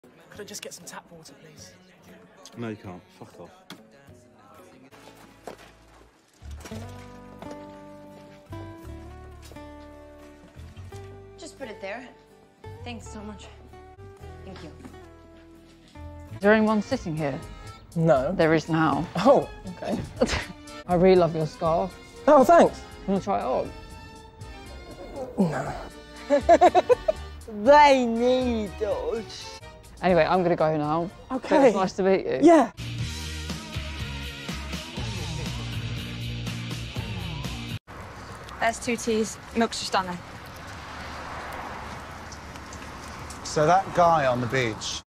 Show Reel - Evan Kenneth Jones
Hi guys!

Here’s a little snippet of a few things I’ve been in.

Ev